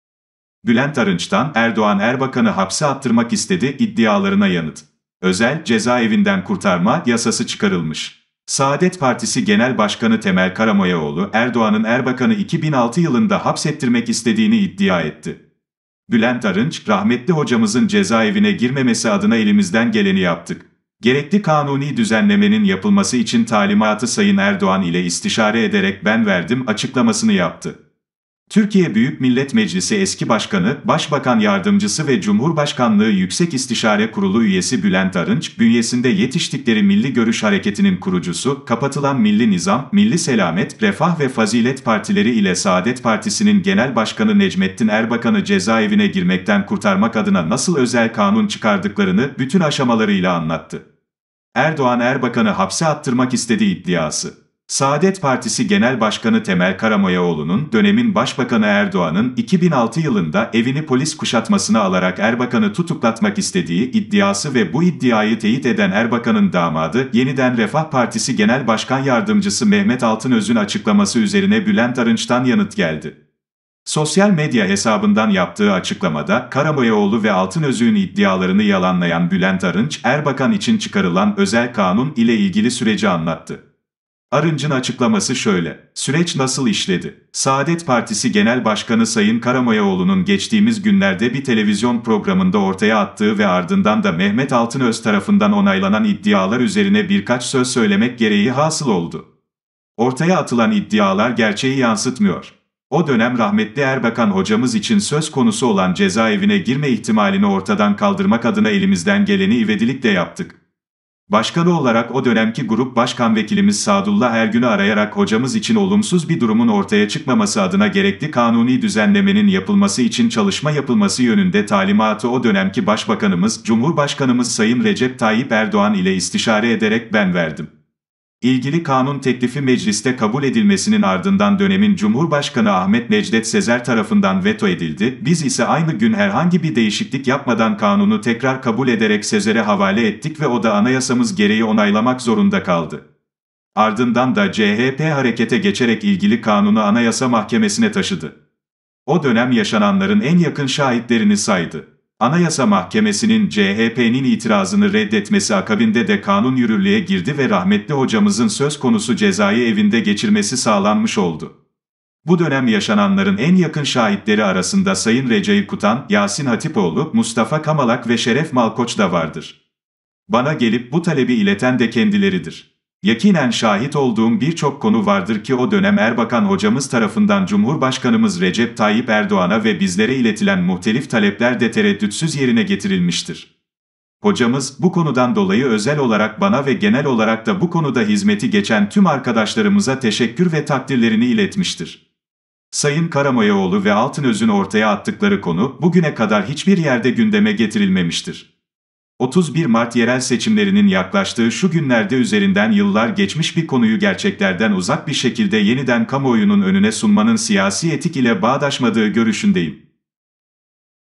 Yıllar Sonra Gelen İtiraf. Erbakan'ı Asıl Kim Hapsettirmek İstedi; Topa Kim Girdi!
Bülent Arınç'tan 'Erdoğan Erbakan'ı hapse attırmak istedi' iddialarına yanıt